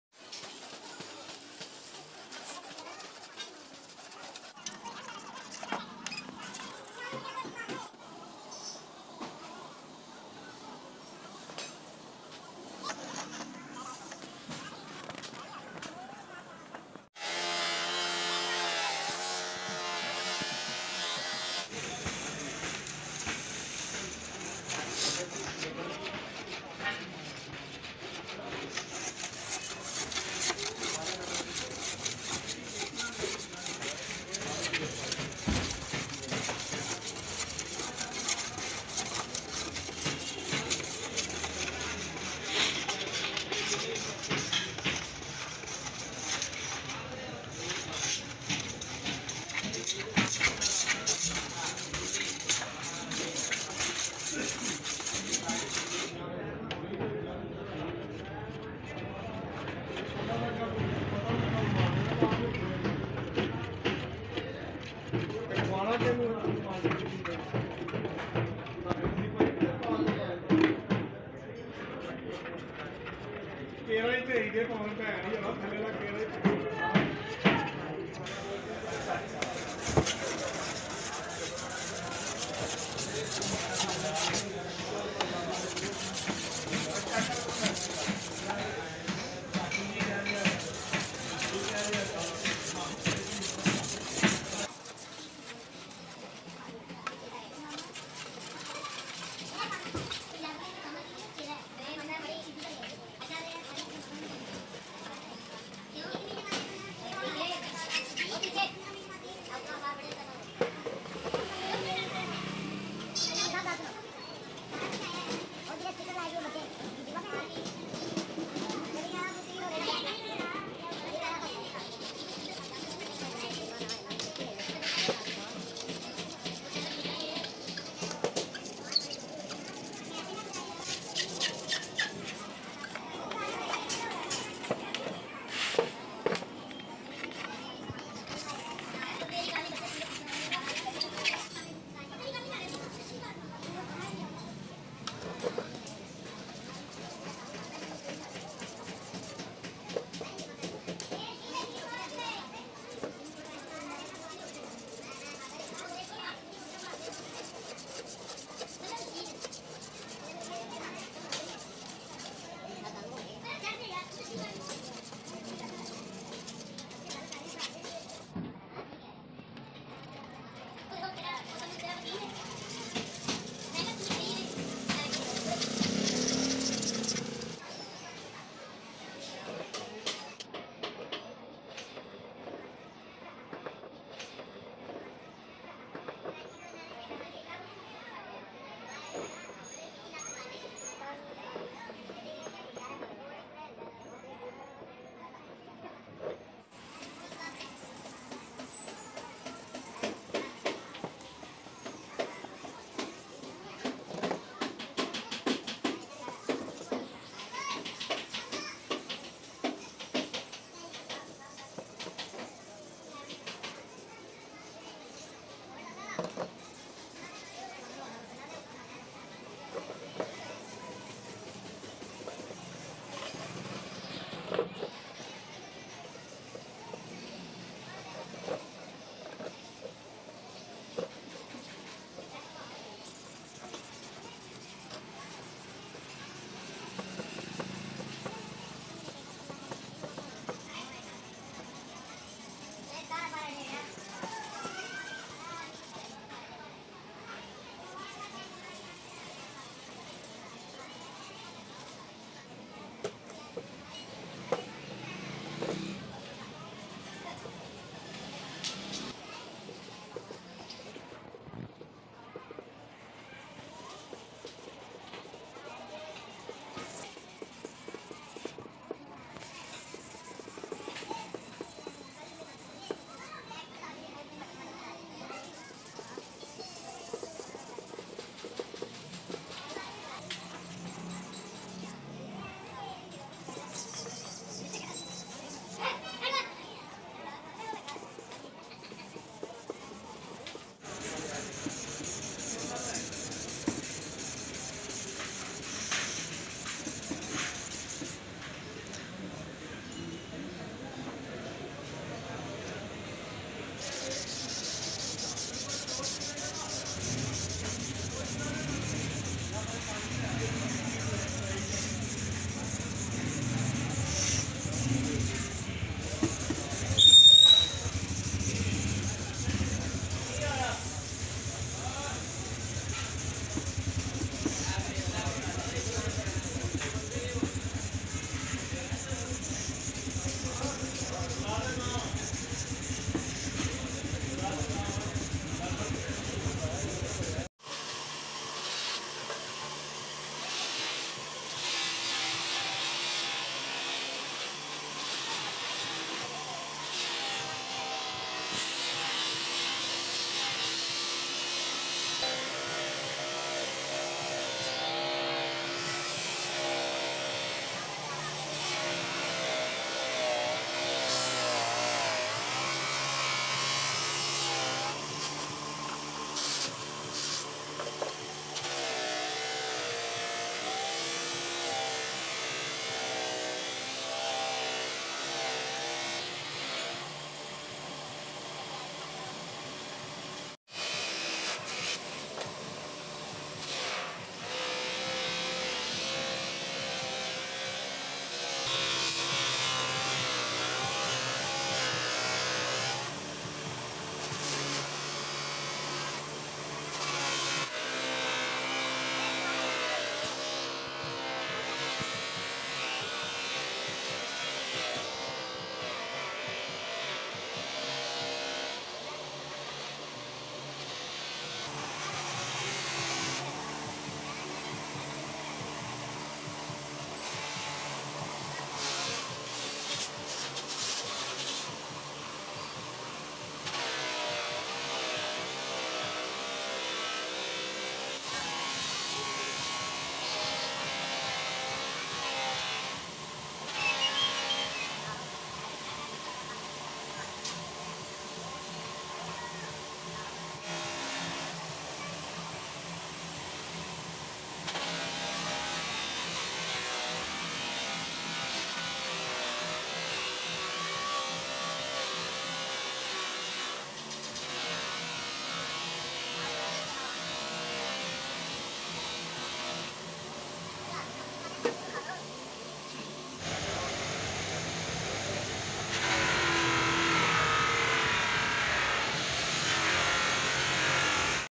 Restoration of Car Headlights | Full Satisfying Restoration video by 10 years old | Handsome skills
10 years old Restoration of Car Headlights | Full Satisfying Restoration video by 10 years old

hey guys this is our new channel and third video hope you guys support us.